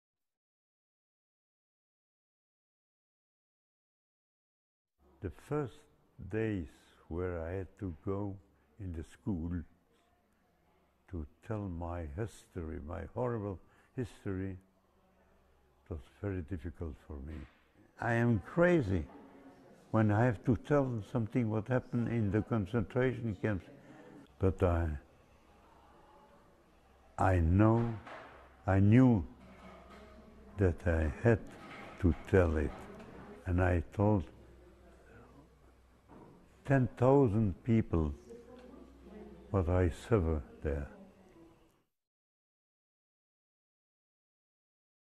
Henri Kichka, has died at the age of 94
Unfortunately Covid_19 has done what Auschwitz could not. One of the last Belgian survivors of the Holocaust, Henri Kichka, has died at the age of 94. RIP you beautiful soul.